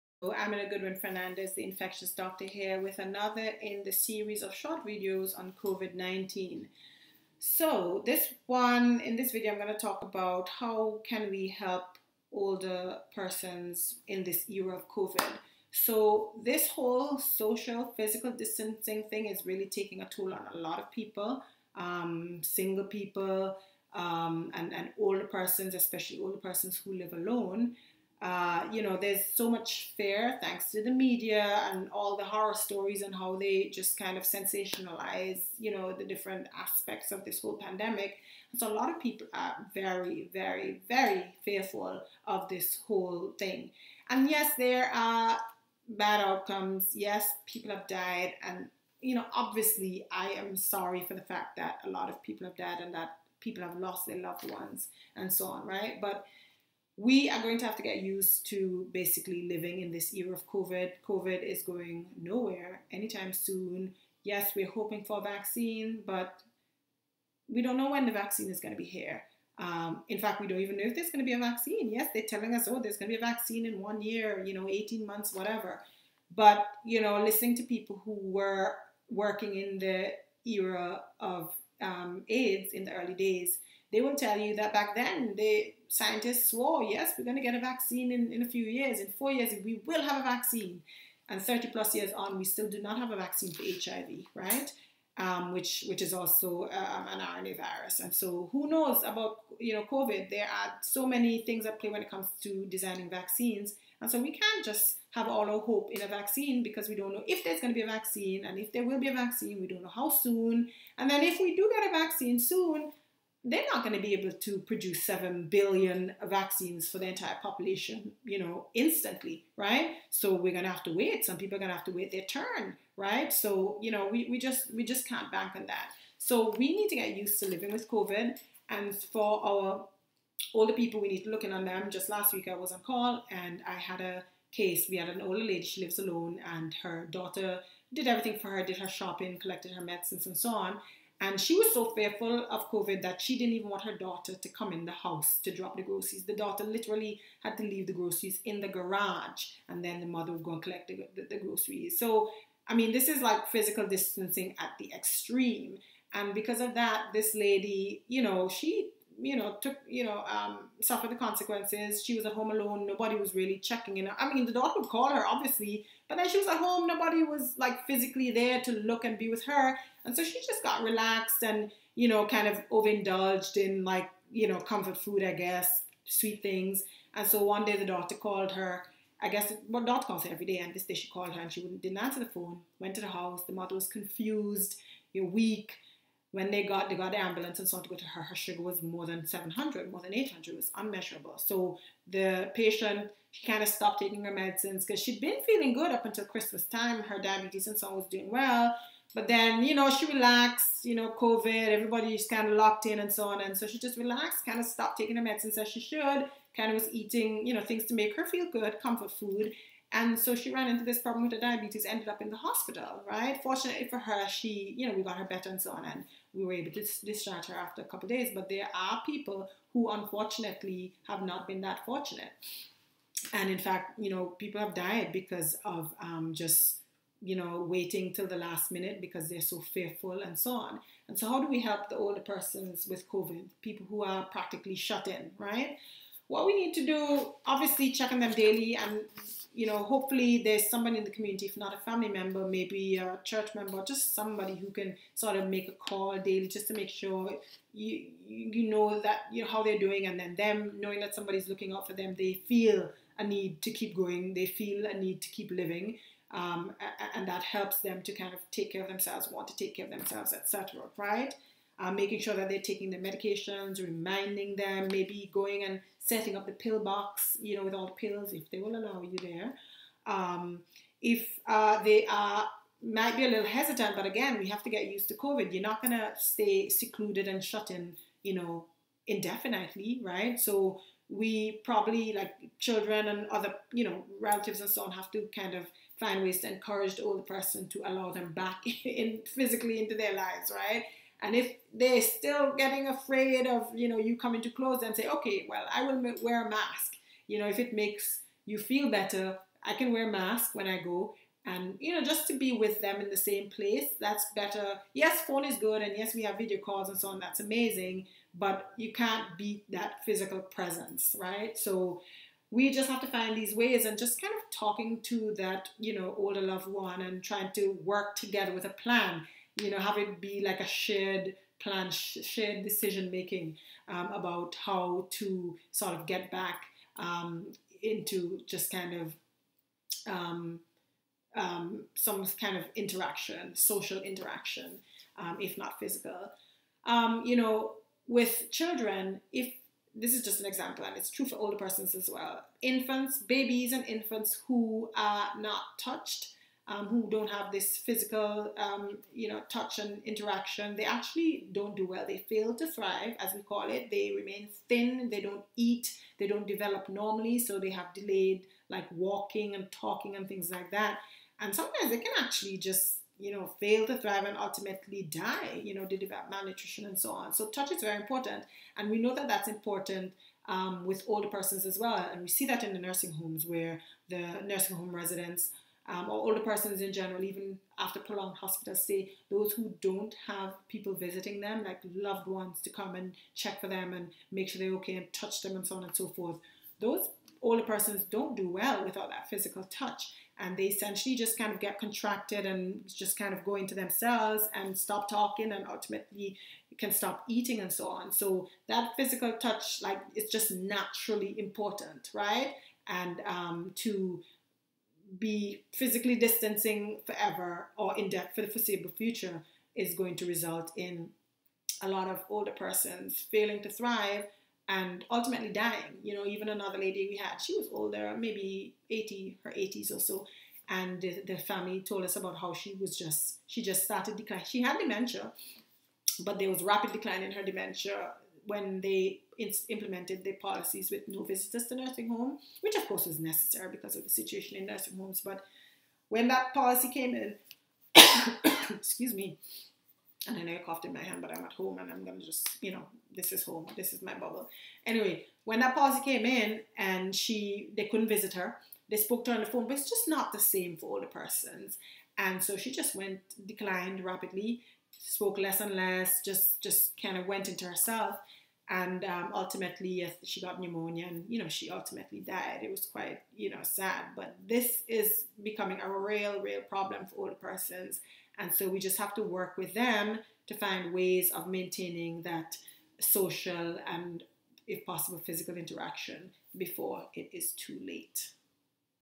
Helping older persons through COVID19
In pediatrics it is well known that touch is extremely important for children. Babies and infants who are not touched - held and cuddled - fail to thrive.

In failure to thrive, children don’t eat well, they are thin, listless, and delayed on their developmental milestones.

This failure to thrive is also seen in elderly persons as well. We see it in nursing home patients who don’t get visitors.

With the new visitation policies in nursing homes brought about by the COVID19 pandemic, failure to thrive is going to become more prevalent. We are already seeing it.

If we don’t find ways to continue to interact with our older friends and family, we will loose a lot of them, not to COVID19, but to failure to thrive.